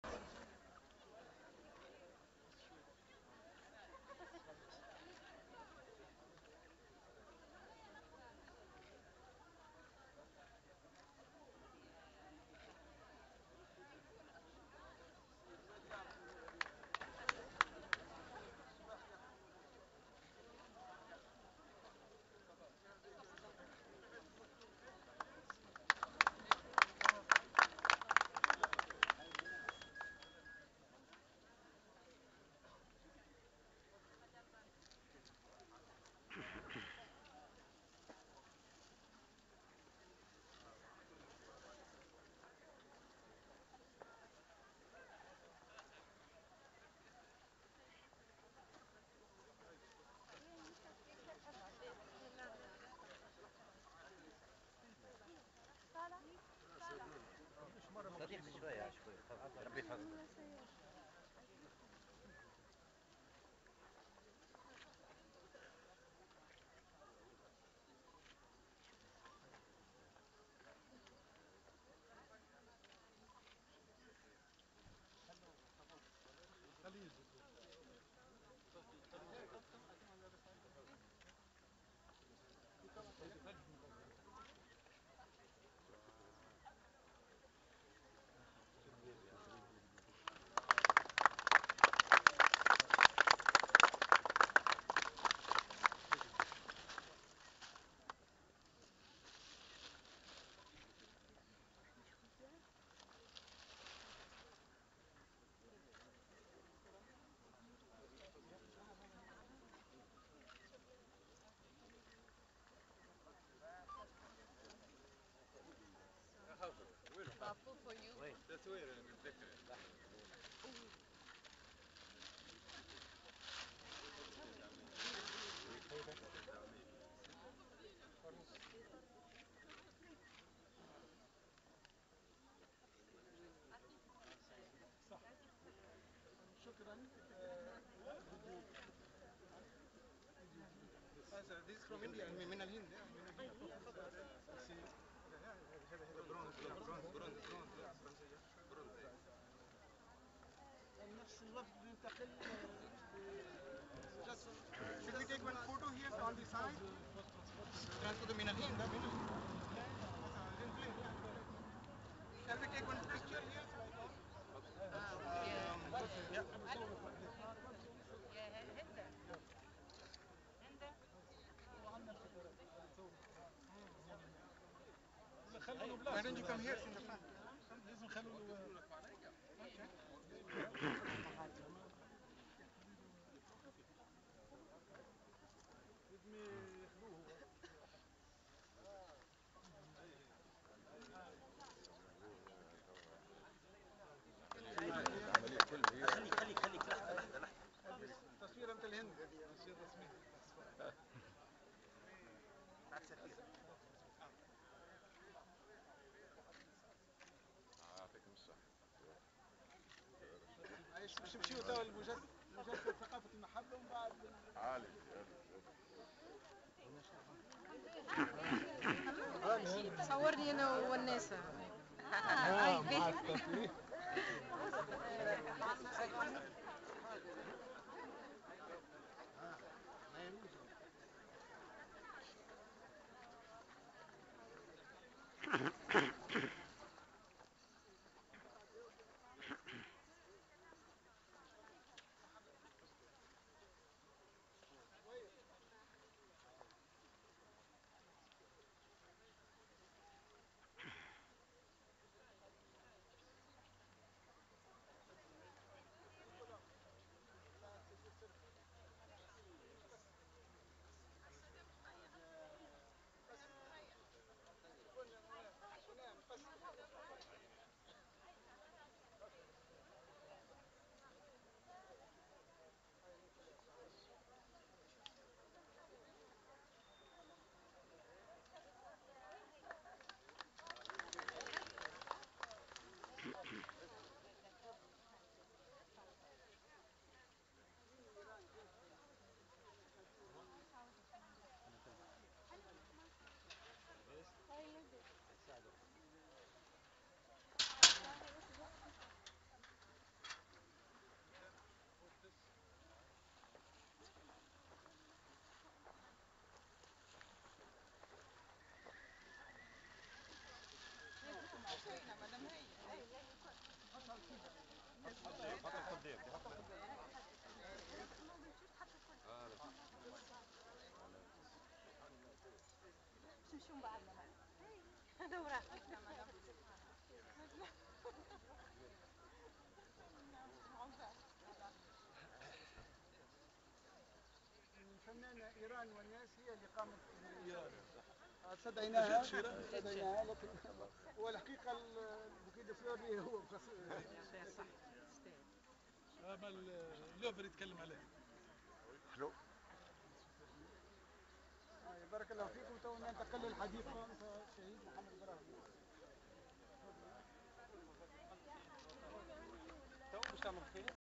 Inauguration des Mémorials à la Faculté des Lettres de Manouba
Inauguration des Mémorials  le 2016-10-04  ,  Jour International  de la lutte contre la Violence à la Faculté des Lettres, des Arts et des Humanités  de Manouba.  Successivement:  Mahatma Ghandi, Mohamed Brahmi et inauguration de la sculpture de Irane Ouaness  "Culture de l'Amour"